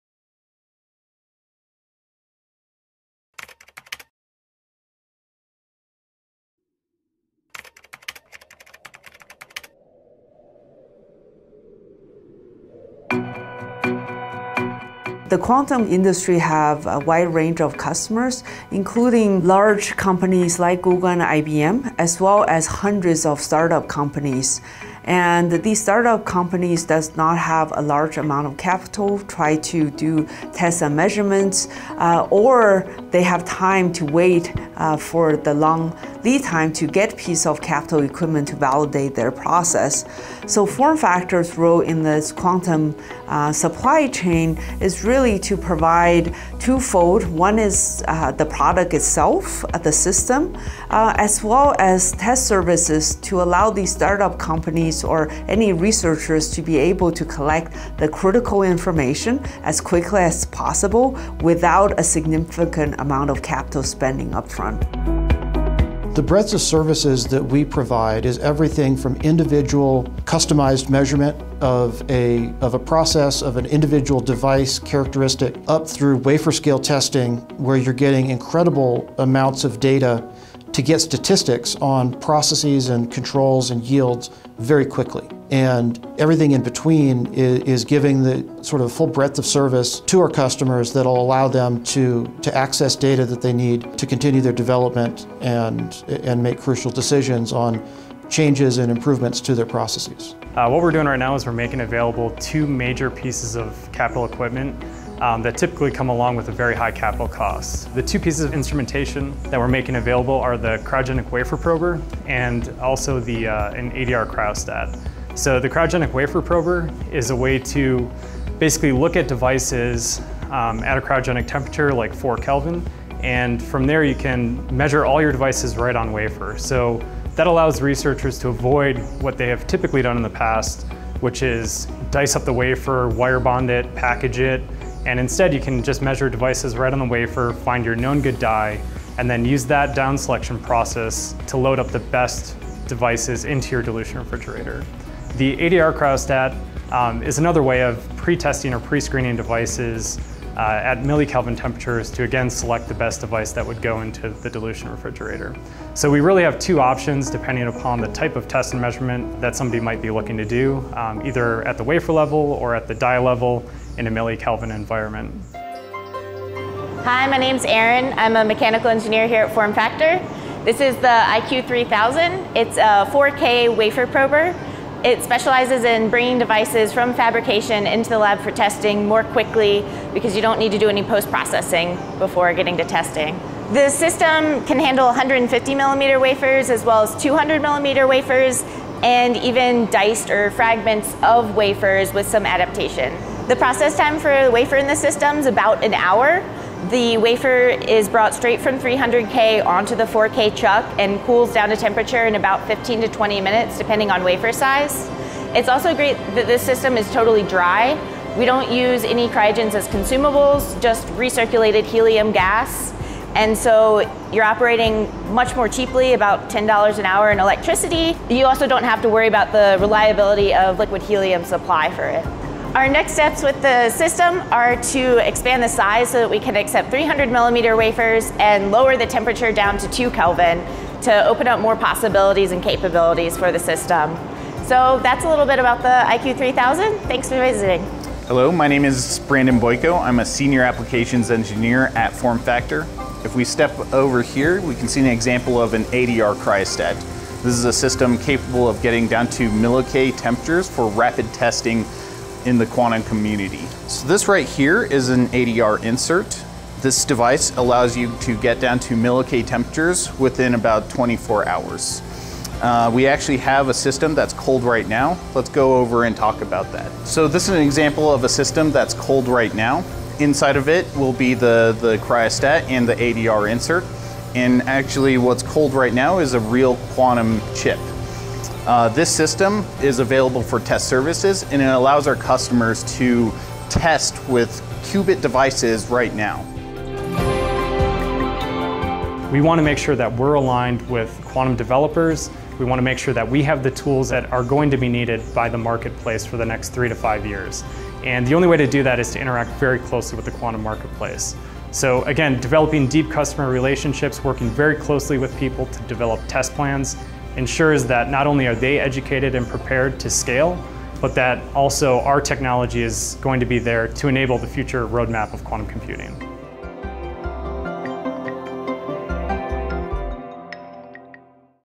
Advanced Cryogenic Test Lab - Cryogenic Test Services | FormFactor
Join us at the Cryogenic Test Lab, located in Boulder, Colorado, where you can partner with FormFactor to build custom probing solutions and collect the valuable cryogenic data you need. Whether it’s at 4 K or less than 50 mK, we will work with you to solve your unique test and measurement challenges.

How can we help enable your technology?
Screen for manufacturing defects at cryogenic temperatures to improve device sorting
Statistically correlate room temperature data with cryogenic performance
Pre-characterize Qubits prior to deployment to reduce QPU bring-up time
Material characterization of superconducting devices
Peace of mind on your next purchase.